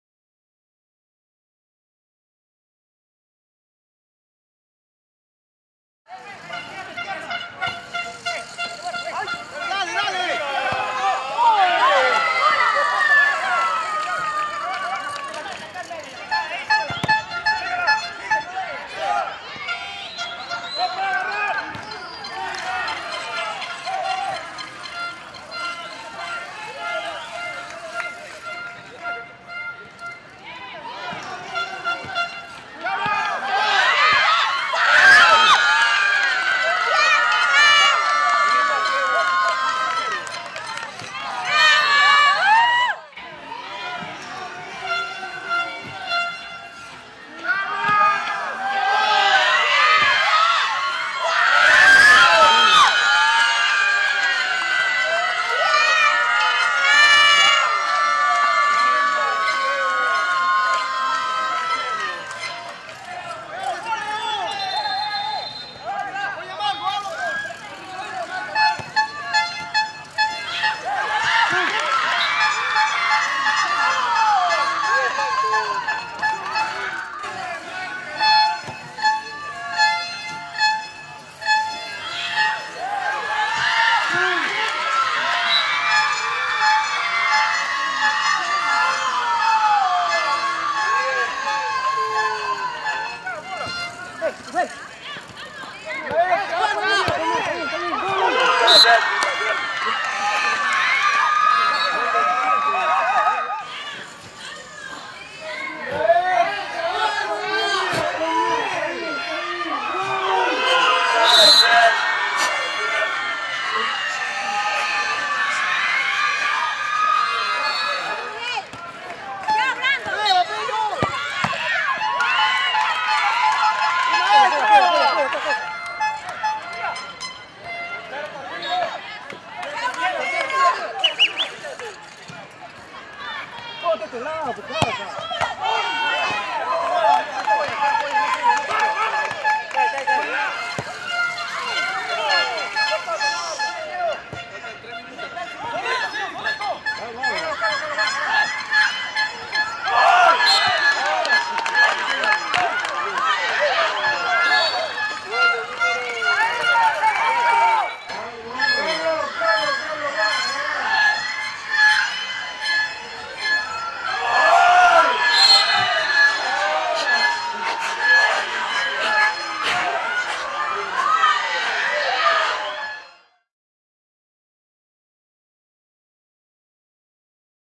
2da. Fecha Copa Confraternidad: Notaría Soldevilla/Jara vs. Notaría Tambini Ávila
Resultado Final:

Notaría Soldevilla/Jara (4) vs. Notaría Tambini Ávila (2)